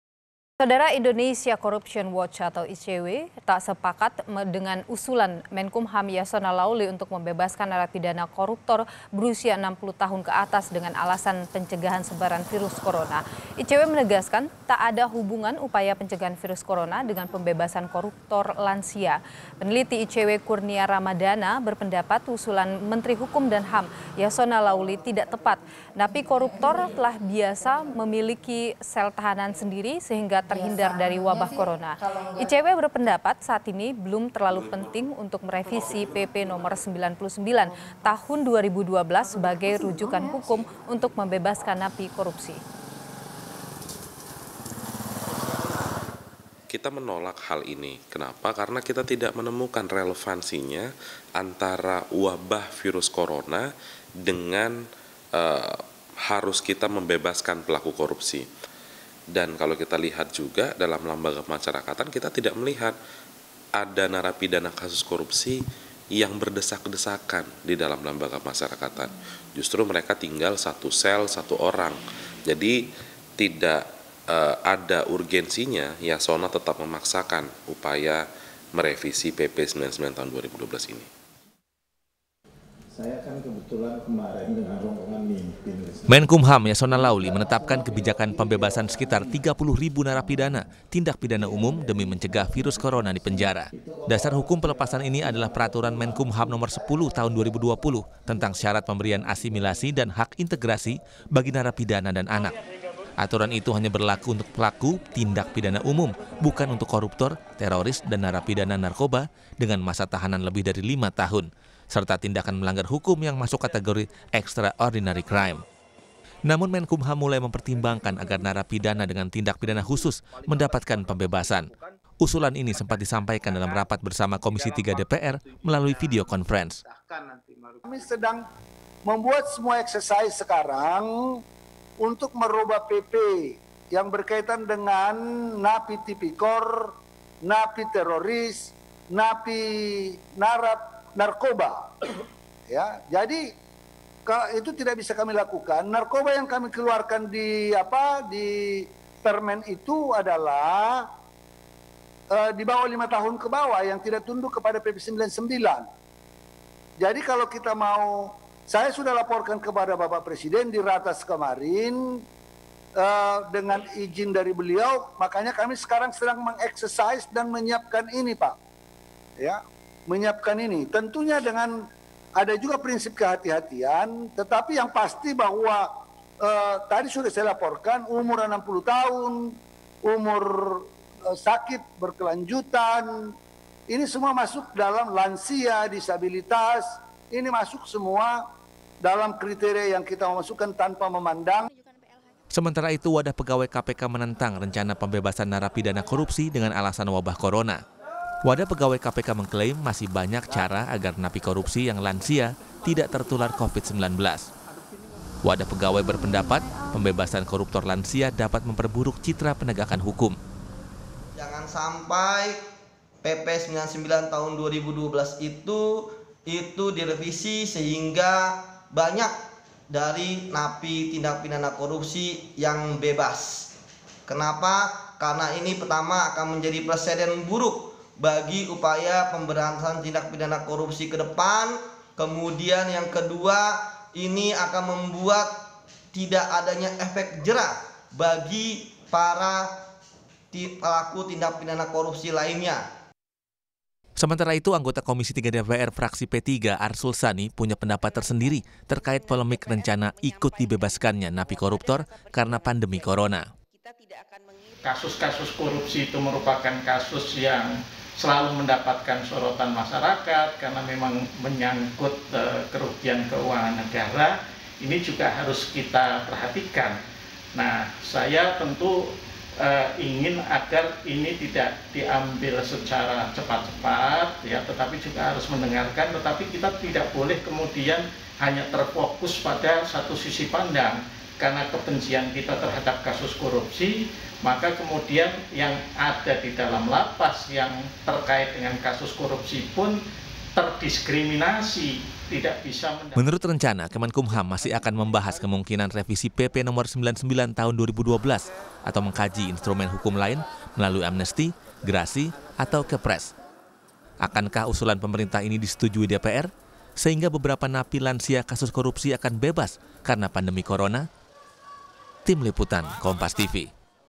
ICW Tolak Usulan Yasonna Laoly Soal Koruptor Lansia Dibebaskan
JAKARTA, KOMPAS.TV - Indonesia Corruption Watch (ICW) tak sepakat dengan usulan Menteri Hukum dan HAM Yasonna Laoly untuk membebaskan narapidana koruptor berusia 60 tahun ke atas dengan alasan pencegahan sebaran virus corona.

ICW menegaskan tak ada hubungan upaya pencegahan virus corona dengan pembebasan koruptor lansia.

\"Kita menolak hal ini kenapa, karena kami tidak menemukan relevansinya antara wabah virus corona dengan harus kita membebaskan pelaku korupsi,\" ucap peneliti ICW Kurnia Ramadhana.

Peneliti ICW Kurnia Ramadhana berpendapat usulan Menteri Hukum dan HAM Yasona Laolly tidak tepat.

\"Kita tidak melihat ada narapidana kasus korupsi yang berdesak-desakan di dalam lembaga kemasyarakatan. Justru mereka tinggal satu sel, satu orang. Jadi tidak ada urgensinya Yasonna tetap memaksakan upaya merevisi PP No. 99 Tahun 2012 ini,\" tambahnya.

Napi koruptor telah biasa memiliki sel tahanan sendiri sehingga terhindar dari wabah corona.

ICW berpendapat saat ini belum terlalu penting untuk merevisi PP Nomor 99 tahun 2012 sebagai rujukan hukum untuk membebaskan napi korupsi.